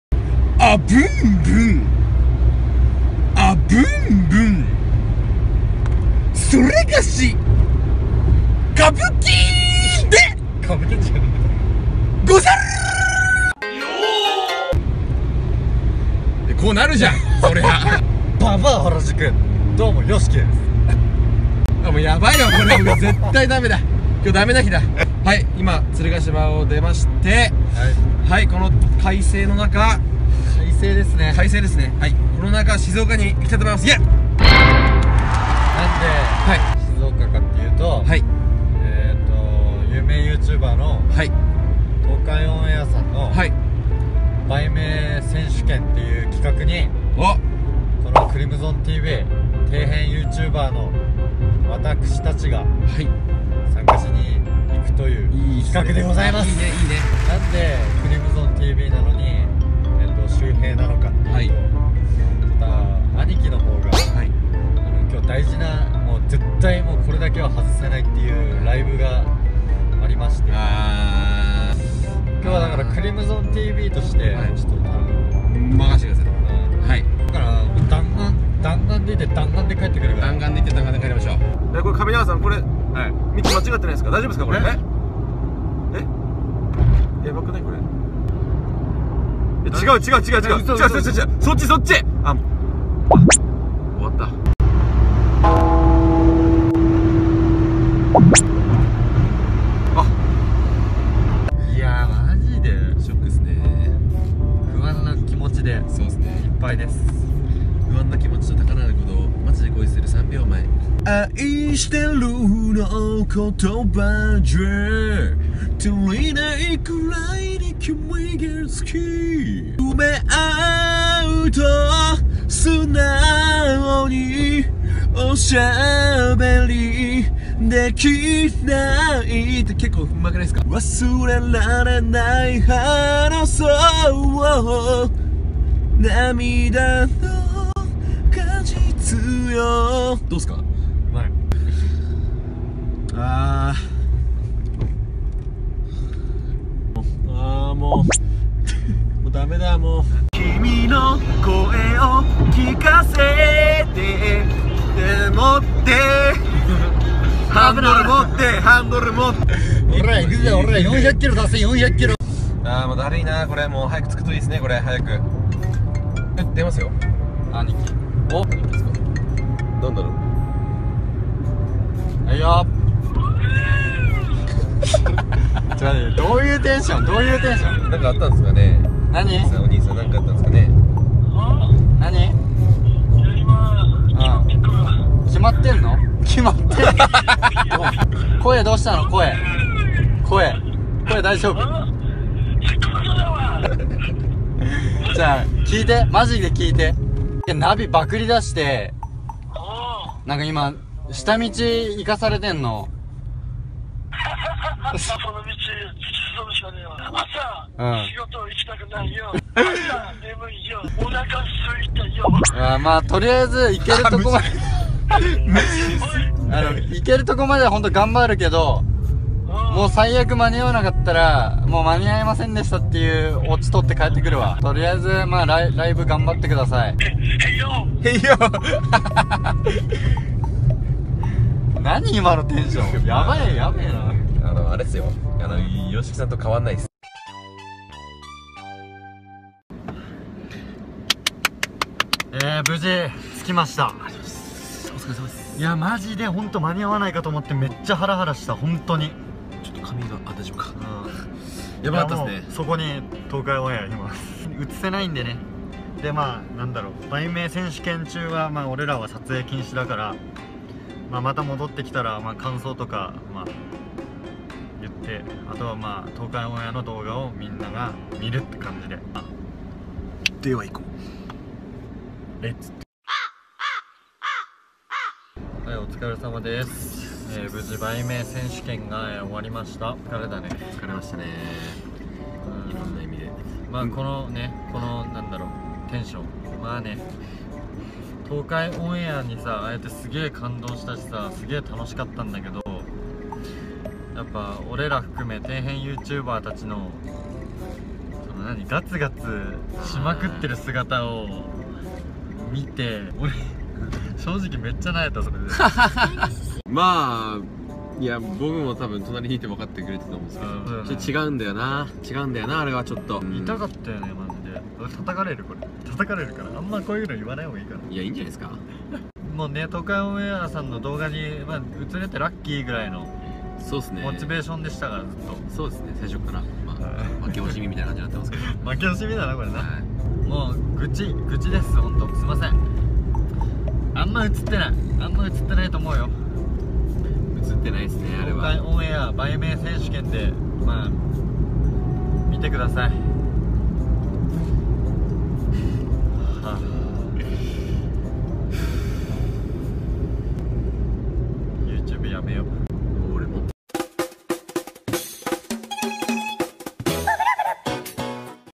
TKDプロジェクトを始めるきっかけとなった動画【東海オンエア売名選手権出場】
約一年前。。この長旅をキッカケにこの2人は今のチャンネルを開設したのであった。
編集もやりとりも初々しいね。